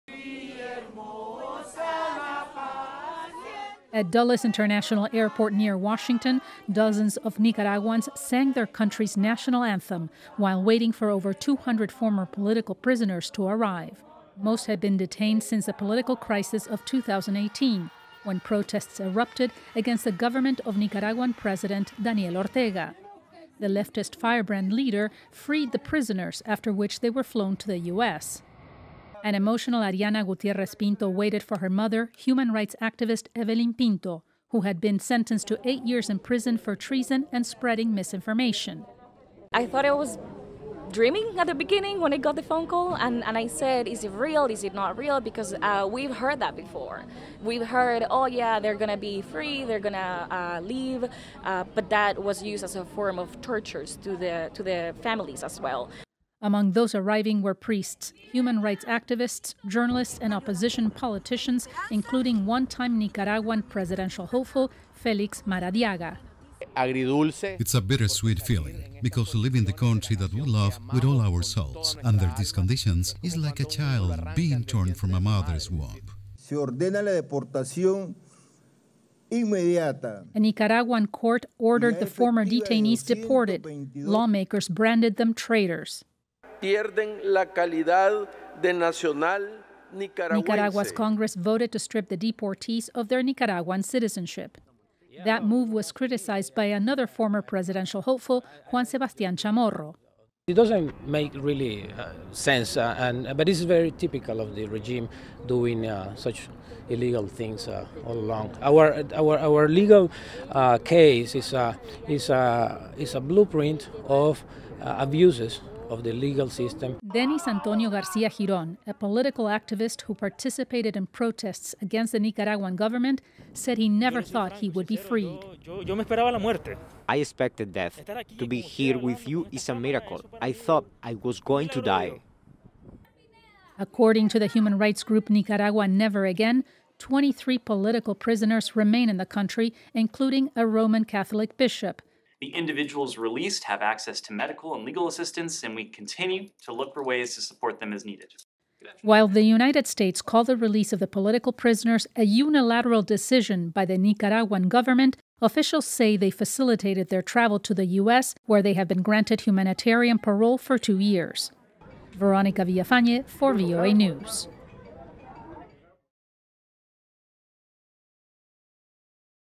Freed Nicaraguan Political Prisoners Granted Humanitarian Parole in US | VOANews
The Nicaraguan government has freed more than 200 political prisoners and expelled them from the country. Many of those released were incarcerated for years for opposing the country’s leadership. All of them have been granted humanitarian protections in the Unites States. VOA’s Verónica Villafañe reports. Camera: Divalizeth Cash, Salome Ramirez, Iacopo Luzi and Donaldo Hernandez Video editor: Veronica Villafañe